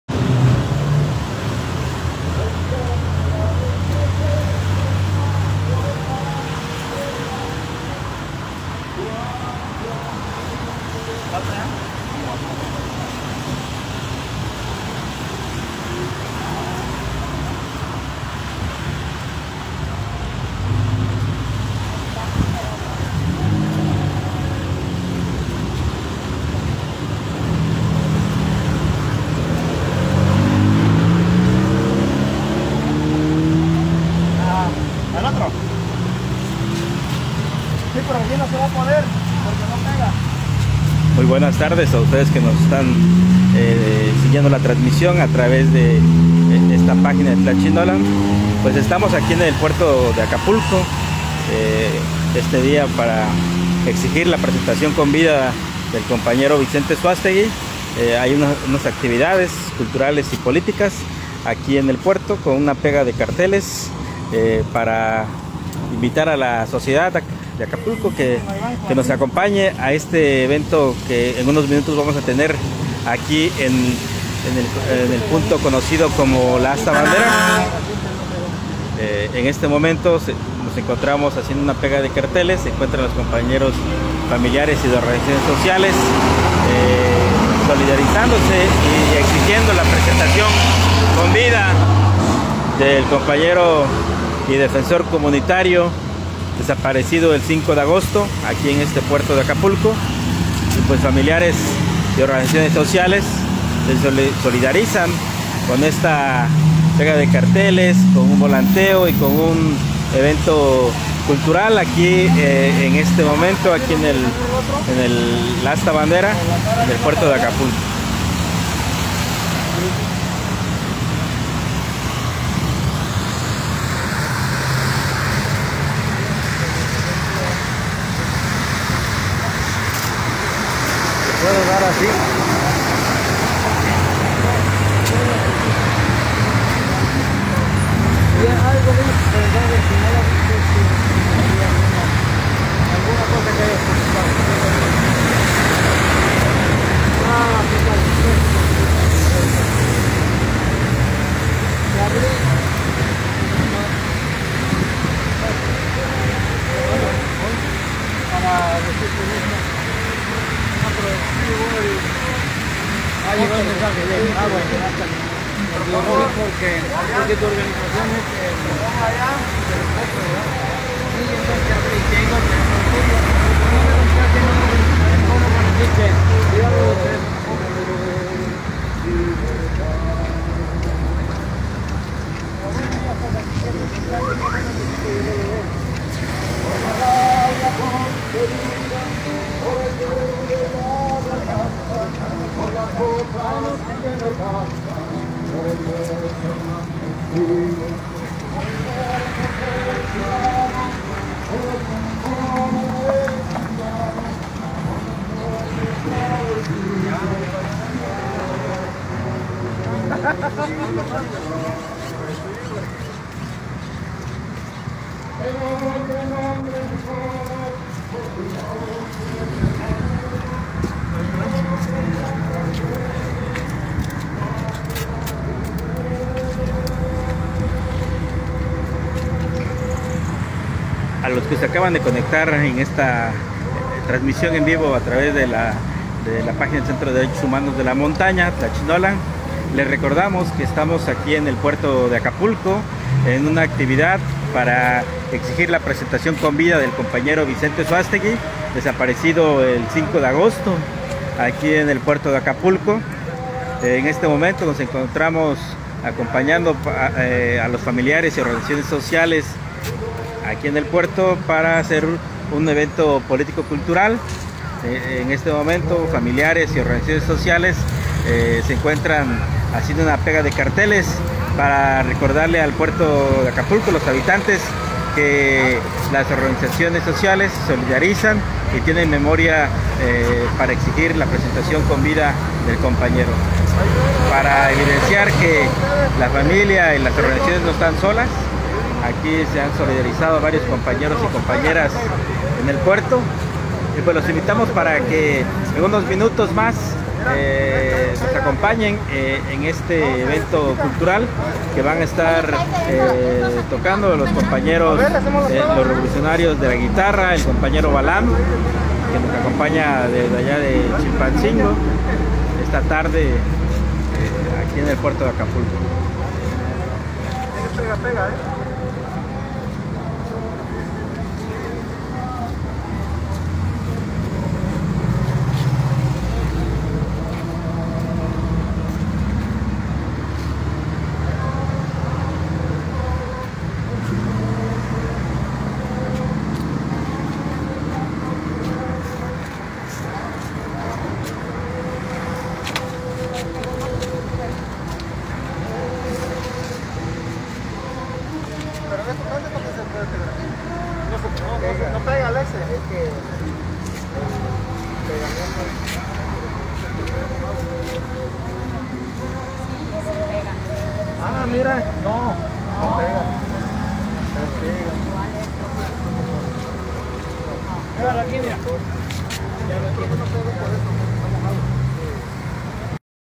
Mitin político- cultural por el defensor de la tierra Vicente Suástegui desaparecido el 5 de agosto
Mitin político cultural por la presentación con vida del defensor de la tierra Vicente Suástegui. Desaparecido el 5 de agosto. ¡¡Porque vivo se lo llevaron vivo lo queremos!!